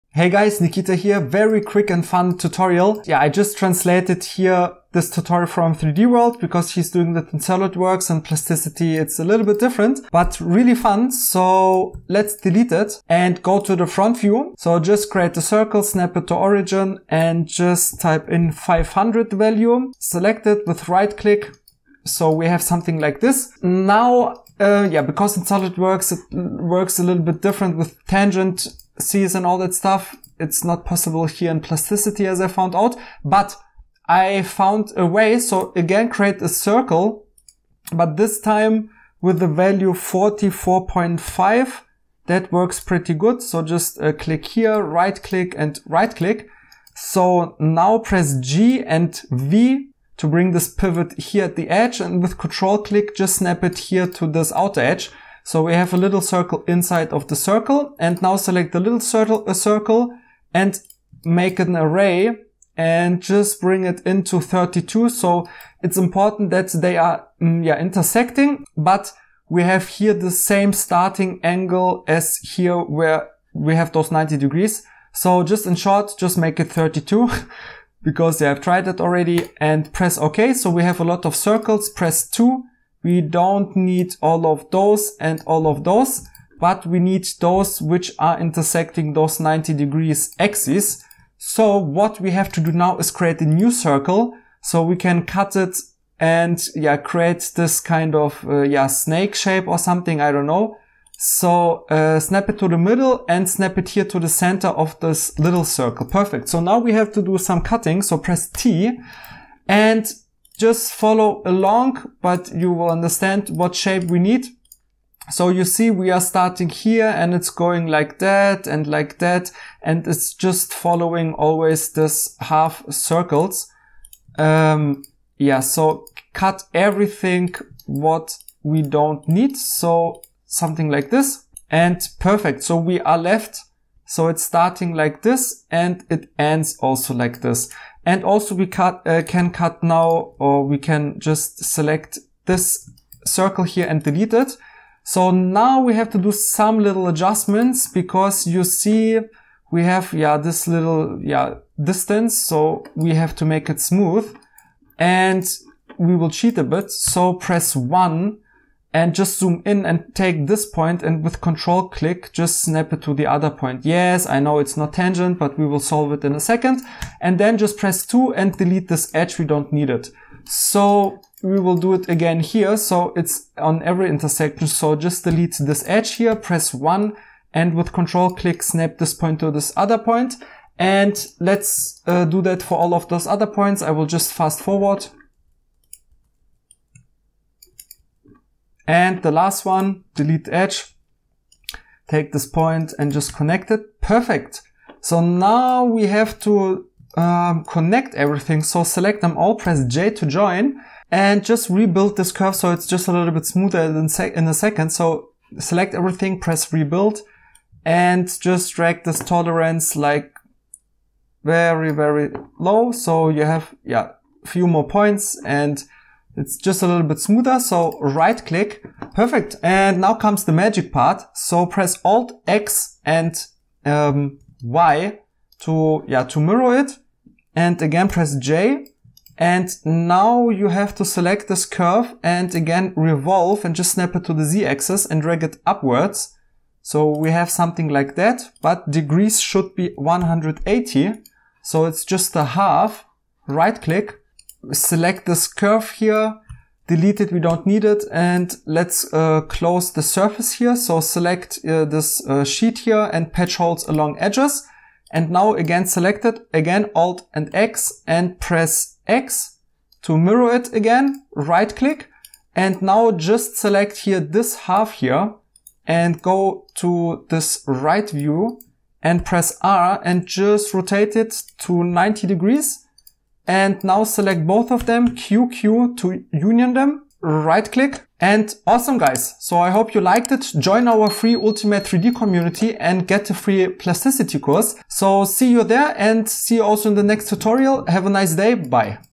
Plasticity 3D | Beginner Modeling Tutorial
📚[PDF Download]
Surface Modeling Guide + Plasticity Shortcuts:

In this concise and engaging Plasticity 3D tutorial i am showing you clever techniques for crafting a captivating, abstract 3D shape.
Learn how to cut, adjust, connect, and mirror elements effortlessly in Plasticity's intuitive environment.

Elevate your 3D modeling skills and unlock new possibilities.

▼ SOCIALS ▼